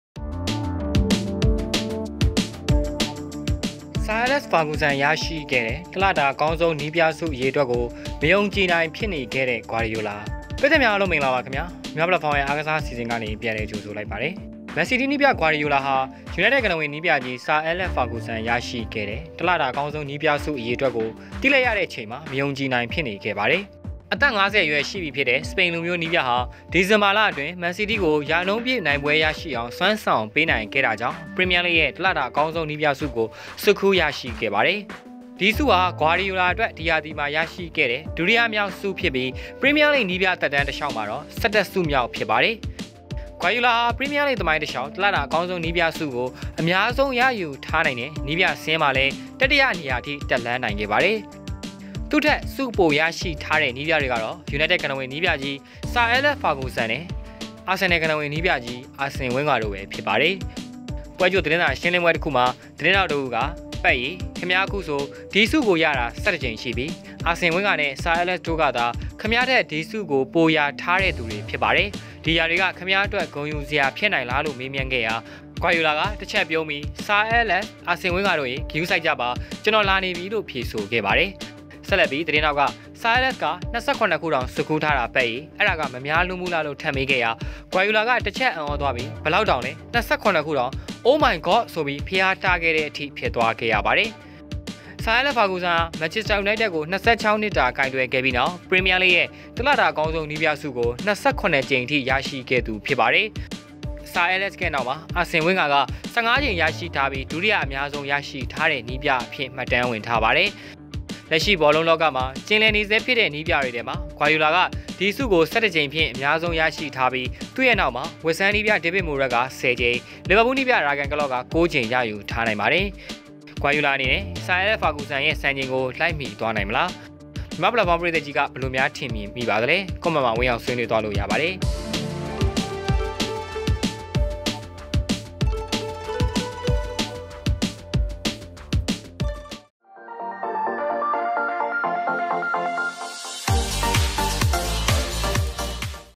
တစ်လတာ အကောင်းဆုံးနည်းပြဆုအရေအတွက်ကို မယုံကြည့်နိုင်ဖြစ်နေခဲ့တဲ့ ဂွာဒီယိုလာ
ဆာအဲလက်စ် ဖာဂူဆန် ရရှိခဲ့တဲ့ တစ်လတာ အကောင်းဆုံးနည်းပြဆုအရေအတွက်ကို မယုံကြည့်နိုင်ဖြစ်နေခဲ့တဲ့ ဂွာဒီယိုလာ